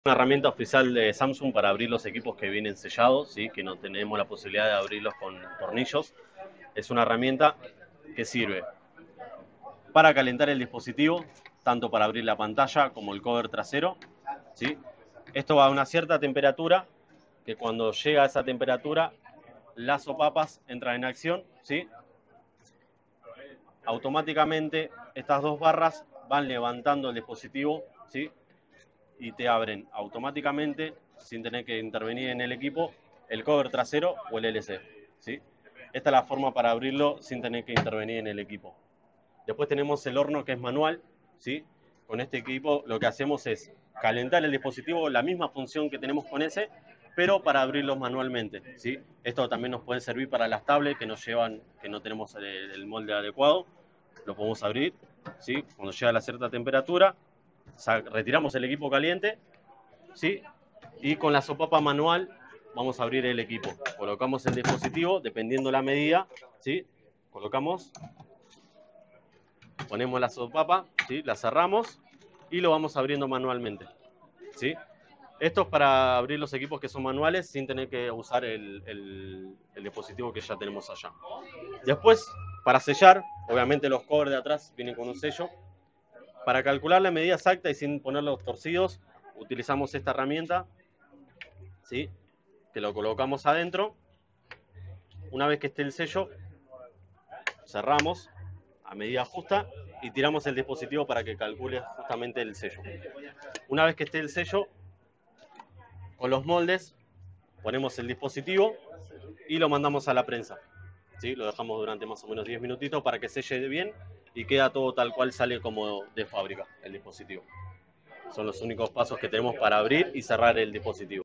Servicio técnico de Compumundo
Recorrimos el RMA que inauguro Compumundo en Argentina. El mismo se llama TechGo.